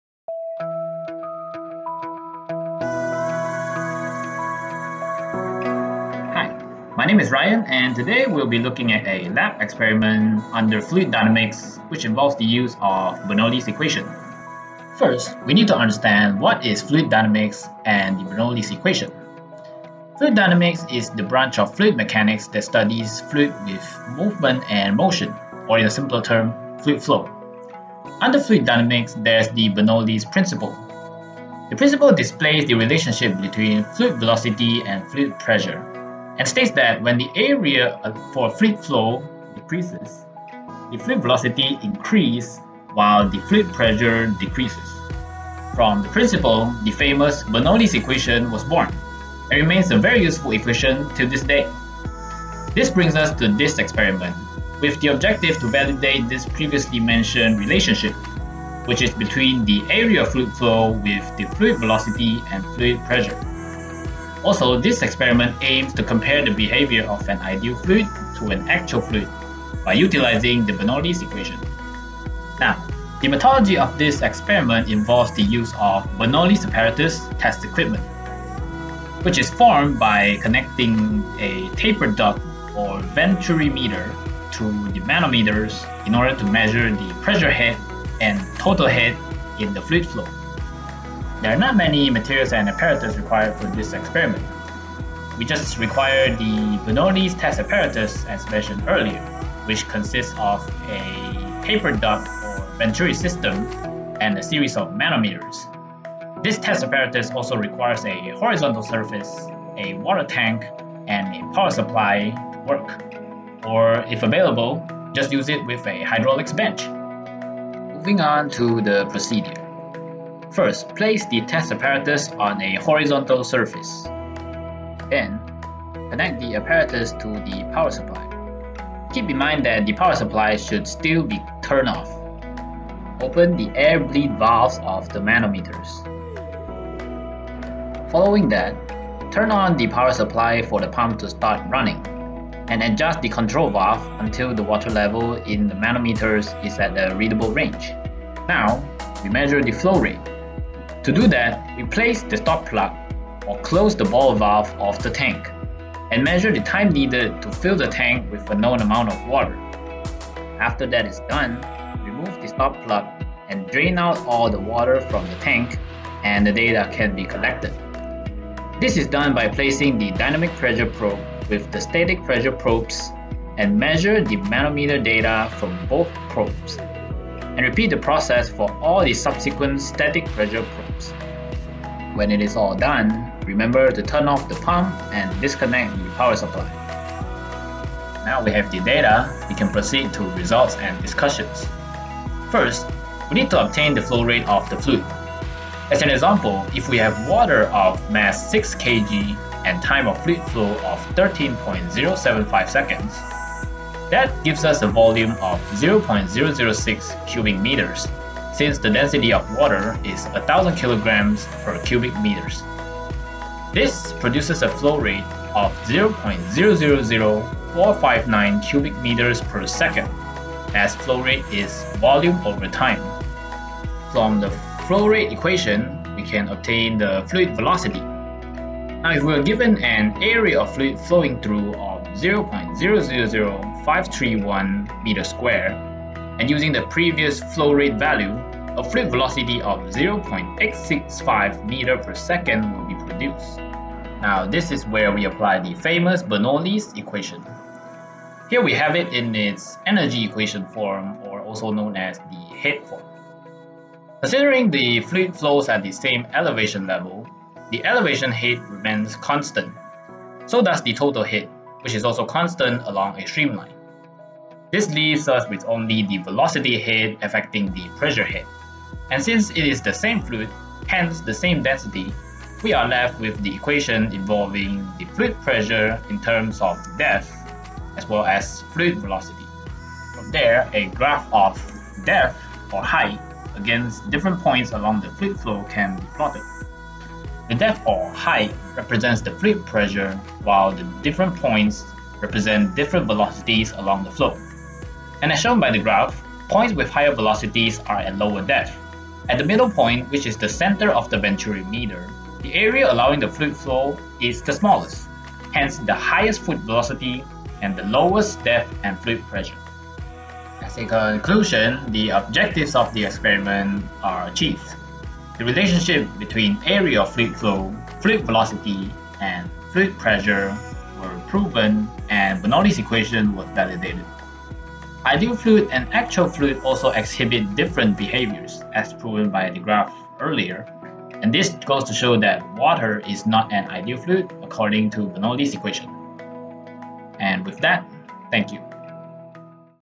MEB 2043 Fluid Mechanics: Bernoulli's Equation
Lab 2 for Fluid Mechanics (MEB 2043)
- Chapter 3 (Fluid Dynamics)
- Produced by Ryan Tee Wei Xi (ID: 18002401)
- Special thanks to:
    * Lecturer: Dr Tuan Mohammad Yusoff Shah Tuan
    * GA: Najat Mohammad Nazari
    * uta.pressbooks.pub
      (EXPERIMENT #2: BERNOULLI’S THEOREM DEMONSTRATION)